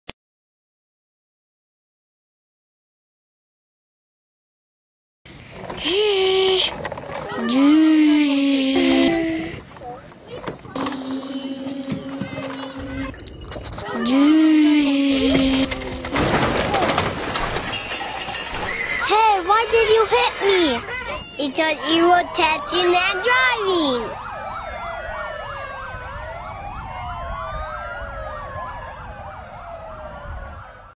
Vincent Soular and Richard Soular act for PSA for City Of Las Cruces Contest
Produced by Ricky Soular.  PSA wins Contest.